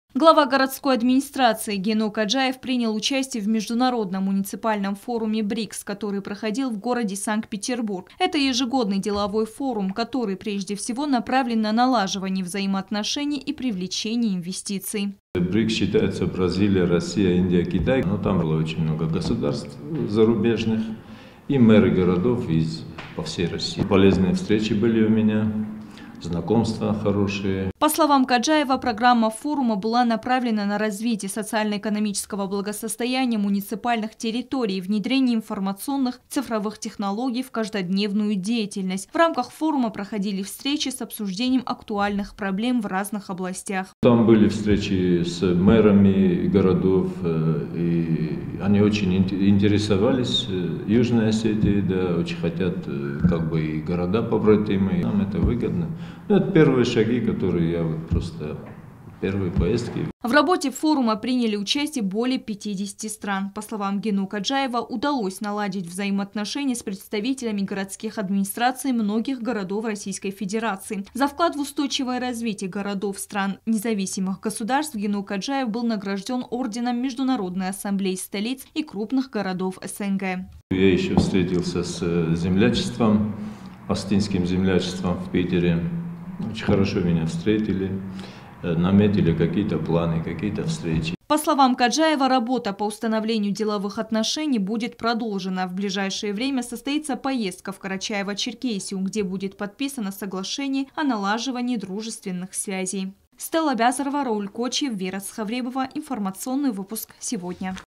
Глава городской администрации Гено Каджае принял участие в международном муниципальном форуме БРИКС.
Четвертый международный муниципальный форум стран БРИКС прошел в Санкт-Петербурге. Площадка объединила представителей из разных стран, среди которых и Южная Осетия. Нашу республику представил глава столичной администрации Гено Каджаев.